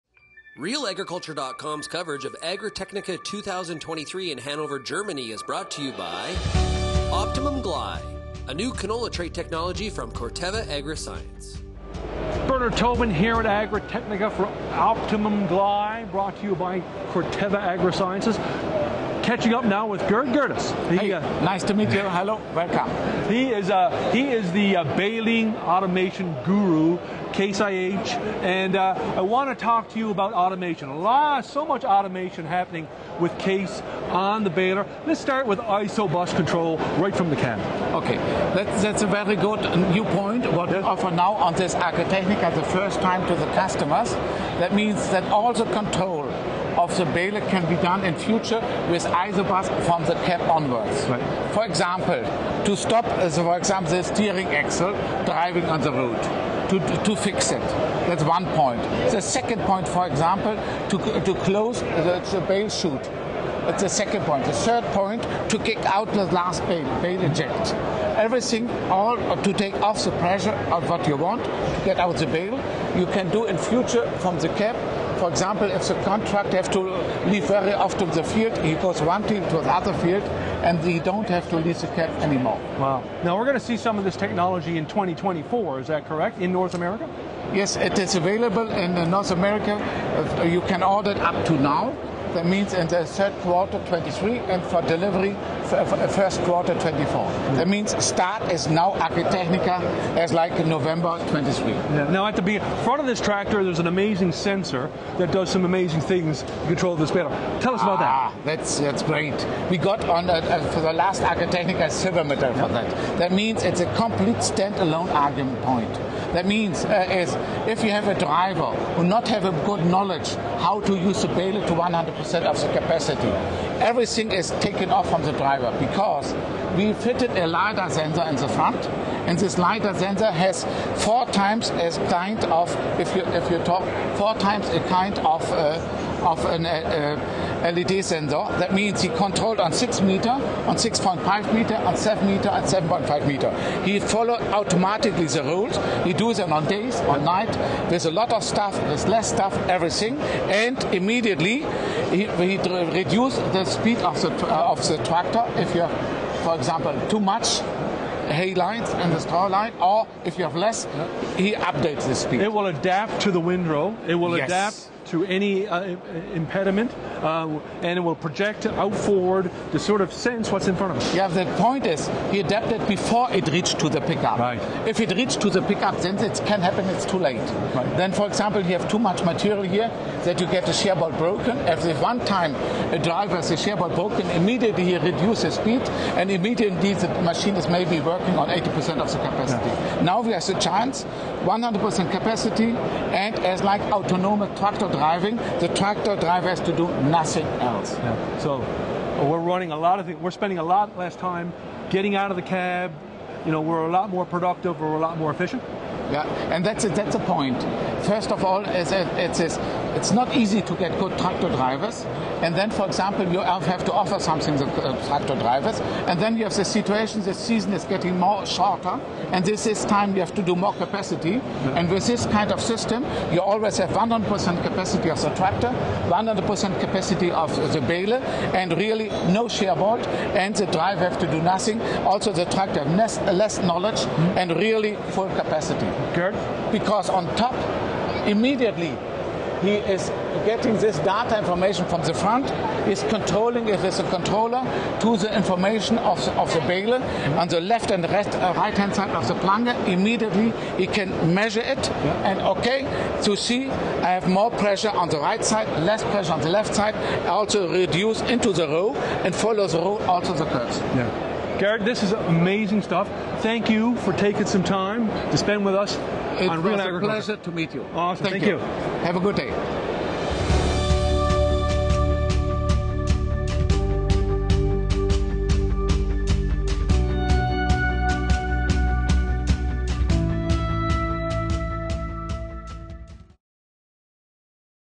Case IH automation moves baler control to the tractor cab
From closing the bale chute to kicking that last bale out of the baler, Case IH, with help from ISOBUS technology, now delivers a higher level of automated baler controls from inside the tractor cab.

In this report from Agritechnica, CNH Industrial baler automation guru Gerd Gerdes shares some of the features North America farmers can expect to see on new Case IH balers for 2024.

Gerdes says a light detection and ranging (LIDAR) sensor mounted on the top of the tractor cab holds the key to many of the new automated controls. The sensor, for example, can scan and determine hay volume in the windrow and increase or decrease tractor speed based on incoming volume to increase efficiency.

Gerdes says advances in baler automation play a key role of optimizing both the tractor and baler during peak production times, with less reliance on operator skills and experience.